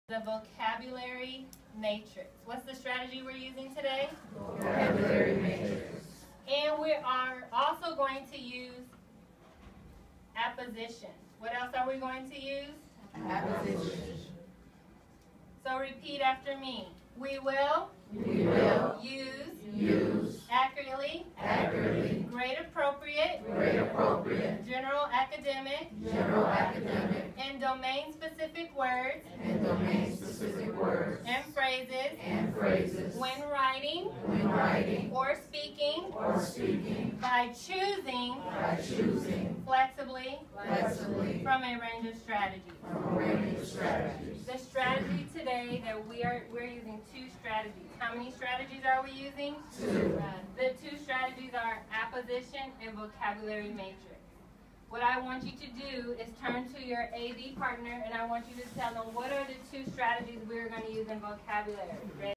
CPS Teacher Professional Development from CEO's Network
This presenter was one of several consultants flown in from California and the United Kingdom for the Chicago Public Schools' Office of Strategic School Support Services' special network. This is a professional development for teachers of Saturday ISAT preparation classes.